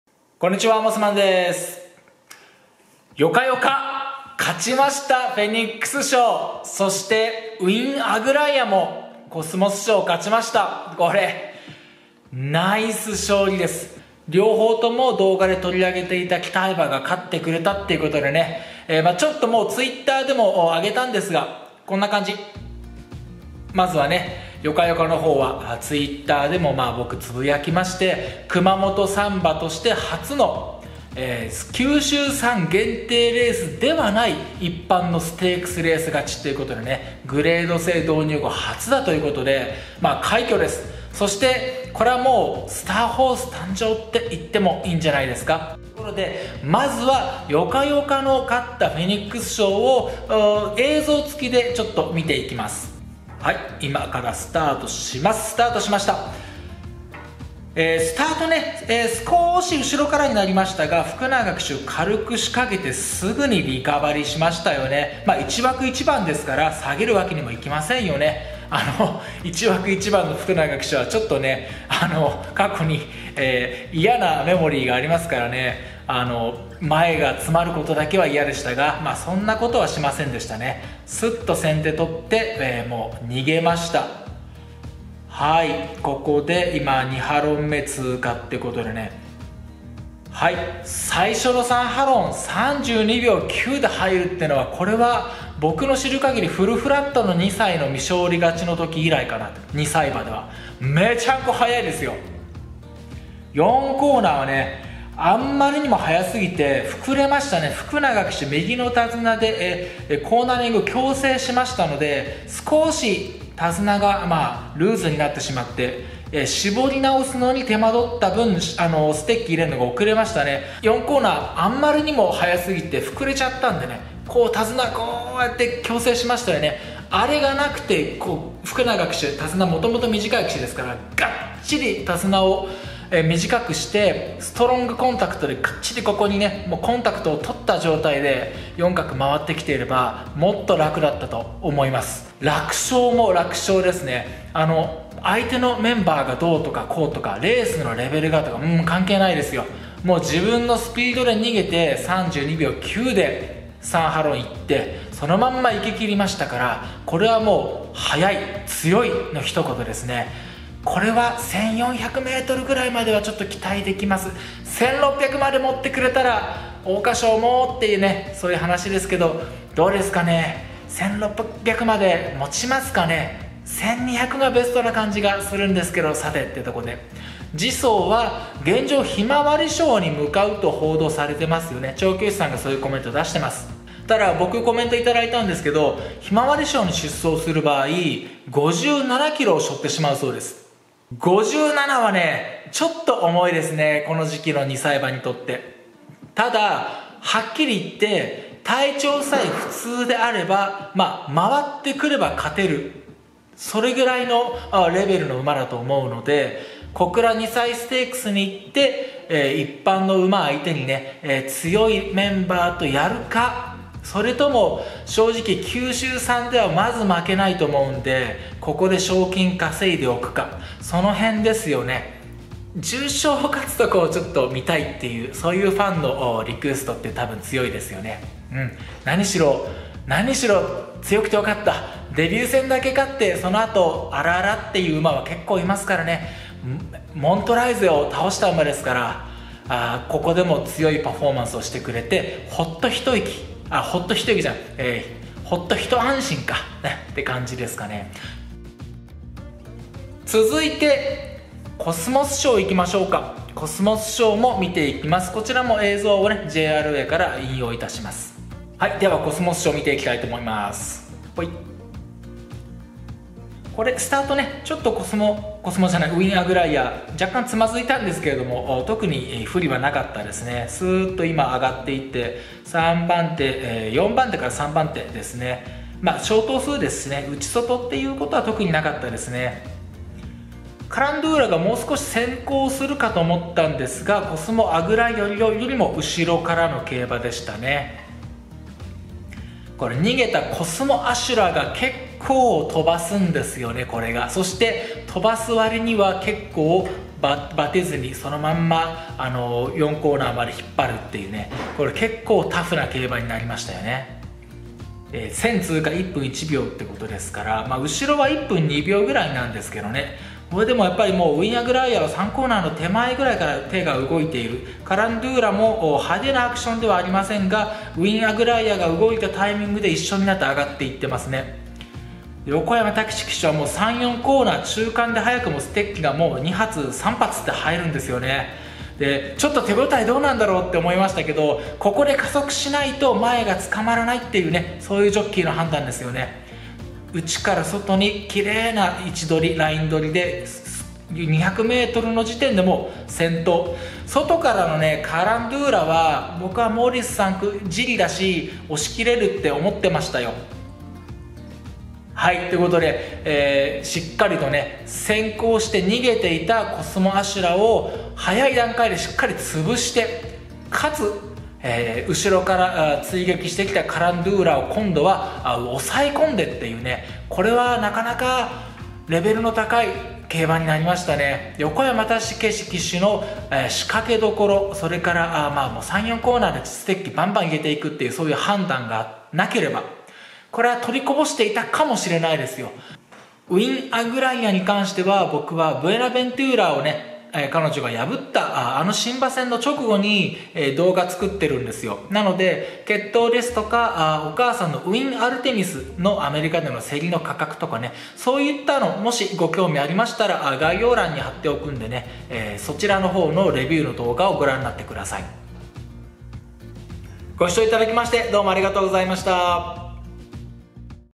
ヨカヨカ速い！ウインアグライア強い！フェニックス賞とコスモス賞のレース解説！
0:00　 モスマンです！
0:53　フェニックス賞レース回顧
4:10    コスモス賞レース回顧

コスモス賞映像　引用先　JRAオフィシャル

フェニックス賞映像　引用先　JRAオフィシャル

Mossman`s equine career：

オーストラリアのシドニーとケンタッキーのキーンランドで
厩舎勤めをしていました。